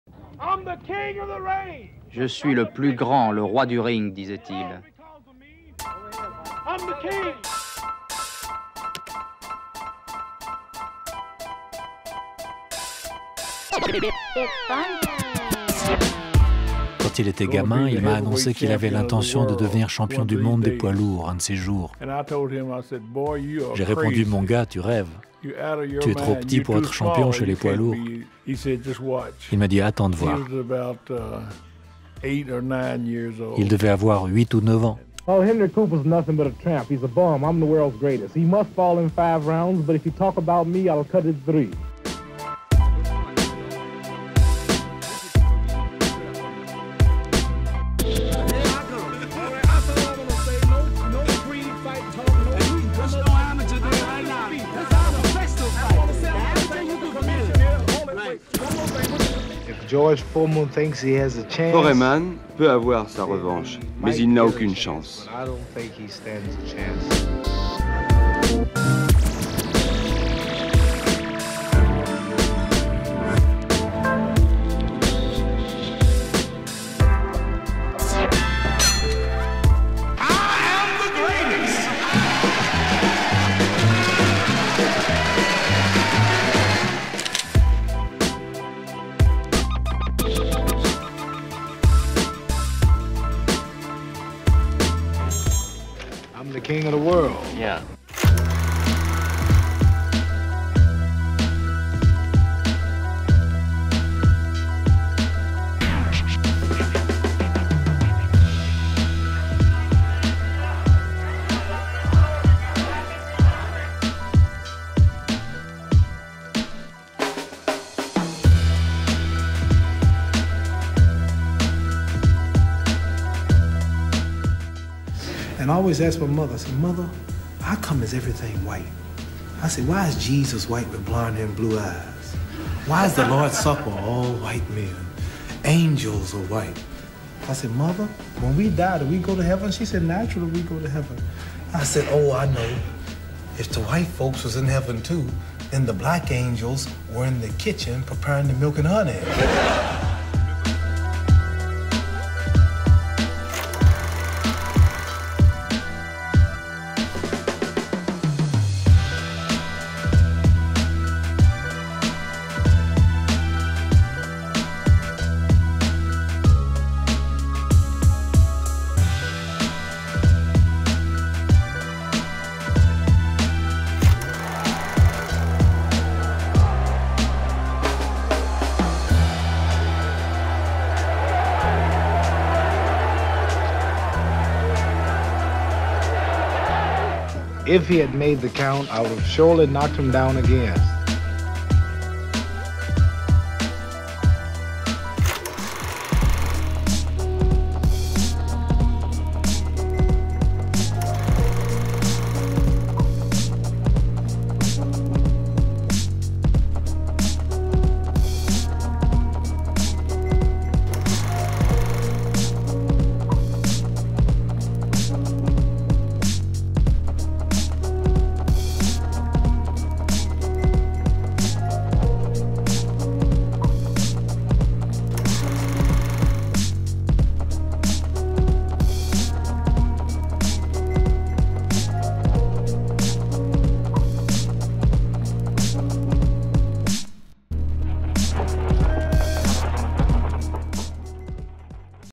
Mohamed Ali : la parole coup de poing
Du "king of the ring" au "king of the world", suivez l'itinéraire de Cassius Clay, devenu Mohamed Ali à 22 ans, alors champion du monde de boxe. Sa parole coup de poing se développe du "trash talking" pour déstabiliser ses adversaires aux provocations politiques et anti-racistes, jusqu'à sa défense d'un islam pacifiste face à Donald Trump. Car dans la vie de Mohamed Ali, le match se joue aussi hors du ring.